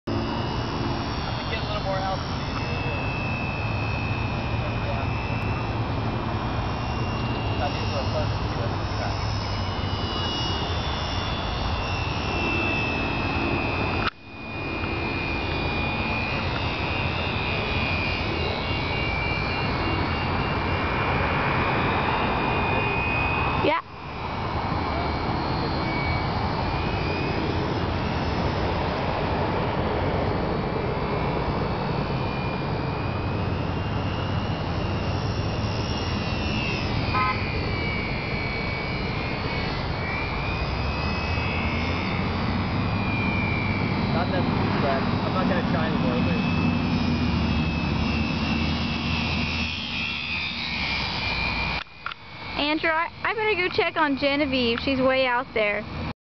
More stalling
Easy way to stall: try loops on a low battery.  This flight is still in my second week of owning the cub.